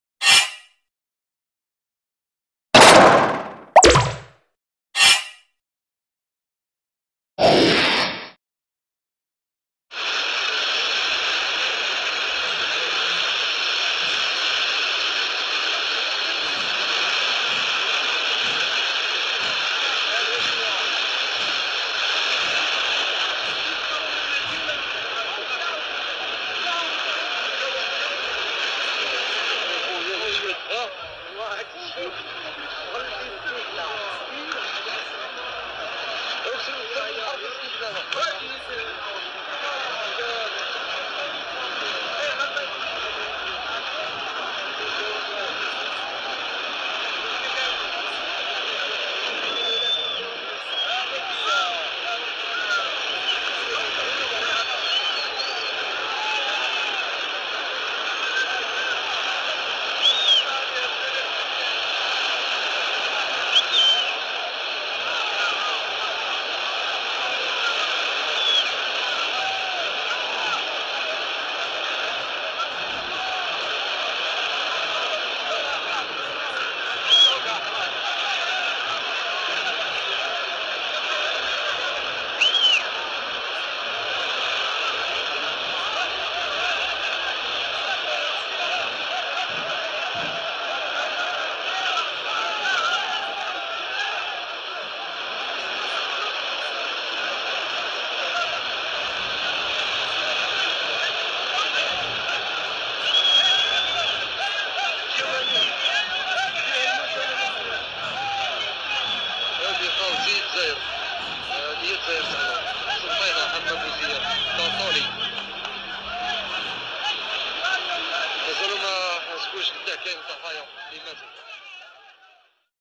فيضانات قسنطينة " ساعة من الأمطار تغرق قسنطينة
فيضانات قسنطينة اليوم ساعة من الأمطار تغرق قسنطينة
توفي شخصان وجرح آخرون،مساء أمس في قسنطينة عقب تساقط أمطار رعدية لمدة لا تزيد عن  النصف الساعة من الزمن والتي نتجت عنها فيضانات عارمة تسبب في كارثة خلفت خسائر مادية وبشرية وغلقا للطرق مما حجز عائلات بأكملها في العراء.